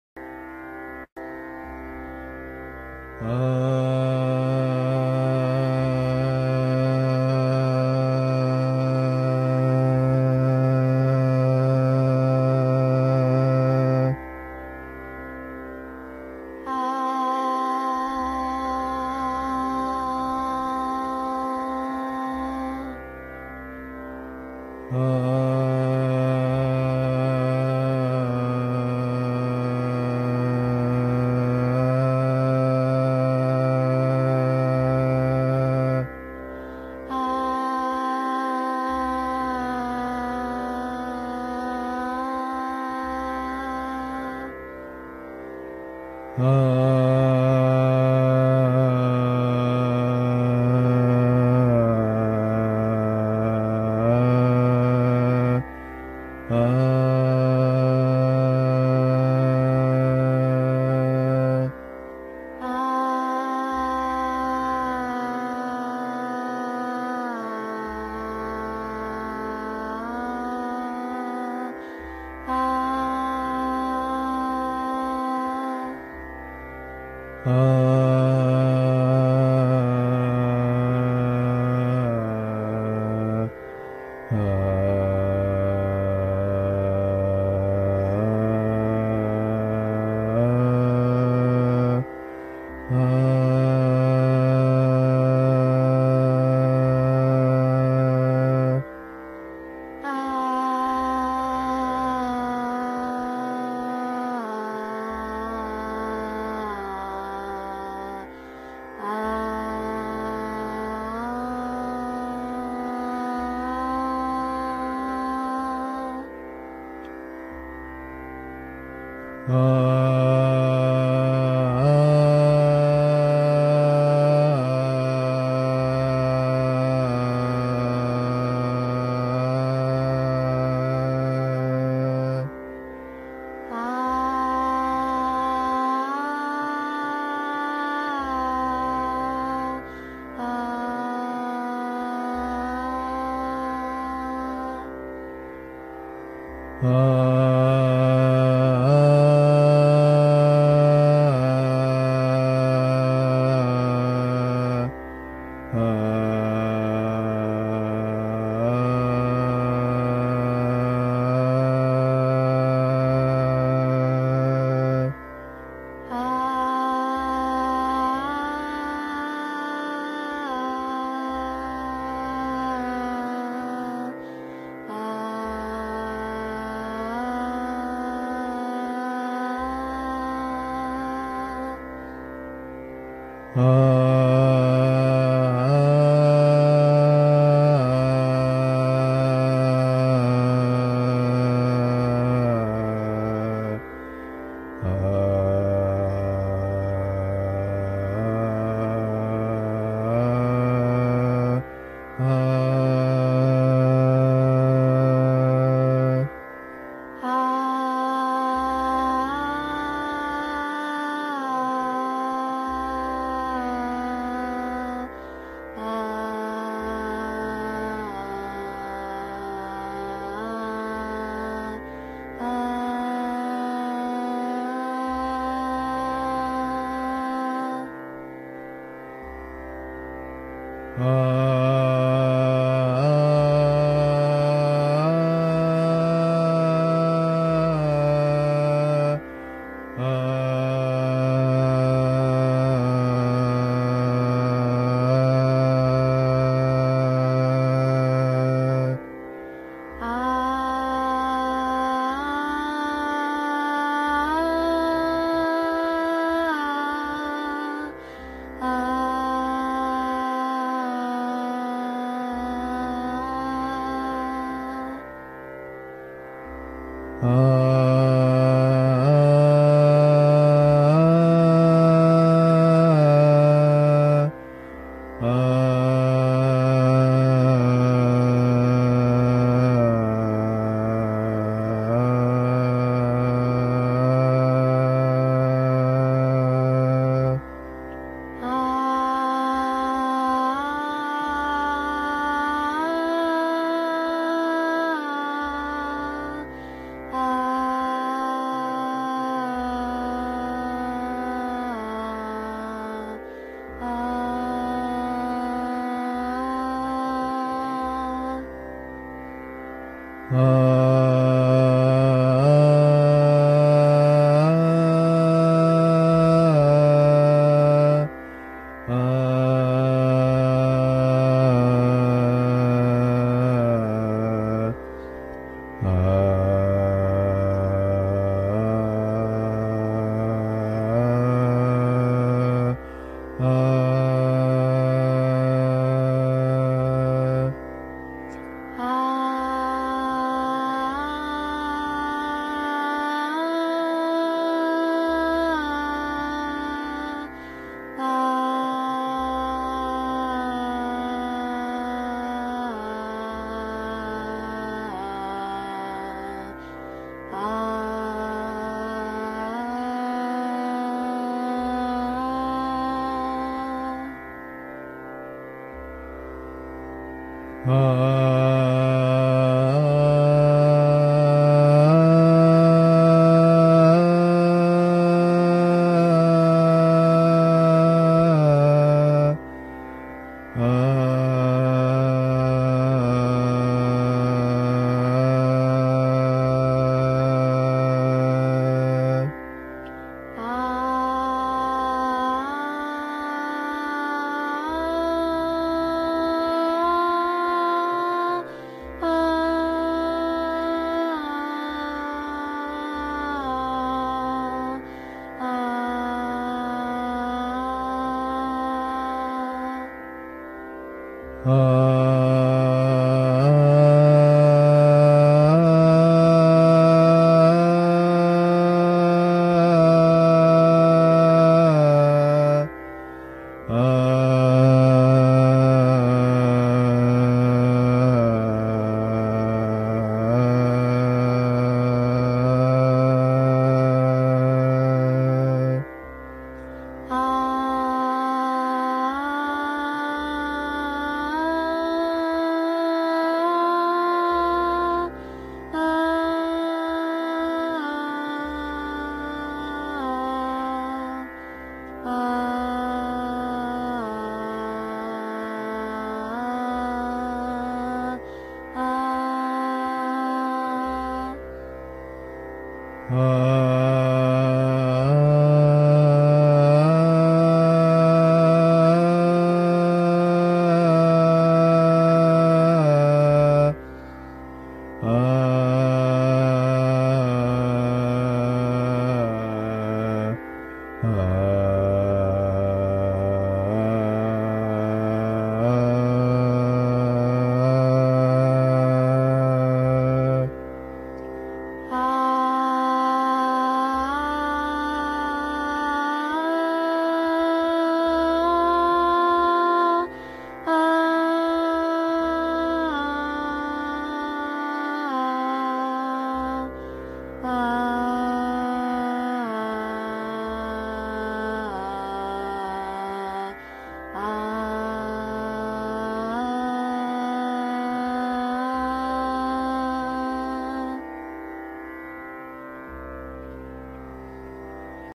Real Voice Lesson 1 Tune Your Voice
Real voice training with alamkar. Listen to Ustad Jee and singalong with female singer and tune your voice. You can singalong without any music instrument or with harmonium or tanpura.

This is real method of voice training. You can also tune your voice with harmonium. Play harmonium notes of thaat bilawal and singalong these notes mentioned in video. Synchronize your voice with harmonium notes of Bilawal thaat or synchronize your voice with female singer and listen to Ustad.